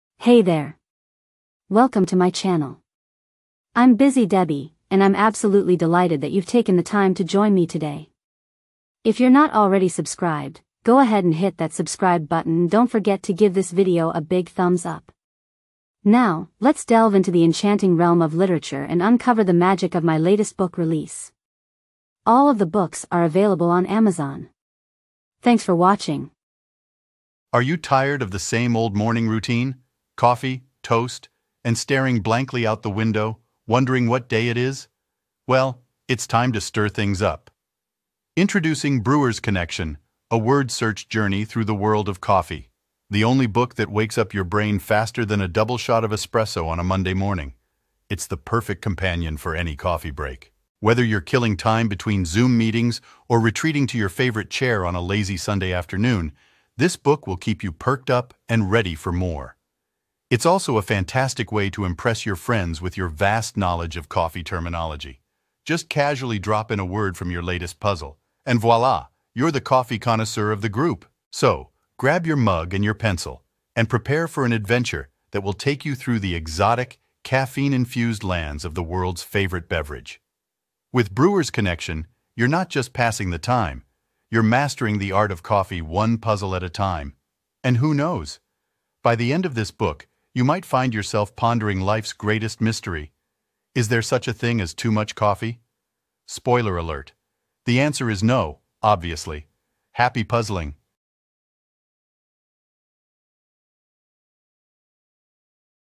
Busy Debbie has a New Book!
Welcome to the official YouTube description for "Brewers Connection: A Word Search Journey Through the World of Coffee," authored by Debbie Danielsen. This isn't just a word search book; it's a voyage into the heart of the coffee world, brewed to perfection for coffee enthusiasts and puzzle lovers alike.

"Brewers Connection" offers coffee-themed puzzles that will challenge your mind and expand your coffee knowledge. From the origins of coffee to the intricate details of brewing techniques, each puzzle is designed to be both educational and entertaining. Dive into the world of coffee through meticulously crafted word searches that cover every aspect of the beloved bean.

Whether you're a casual coffee drinker or a seasoned barista, this book is your ticket to a deeper appreciation of your daily cup. It’s perfect for anyone looking to pass time, stimulate their brain, or just enjoy a fun and unique challenge with their morning coffee.

Grab your copy today and join the worldwide community of coffee aficionados who are already enjoying their journey through the world of coffee with every page they turn. Happy puzzling and happy brewing!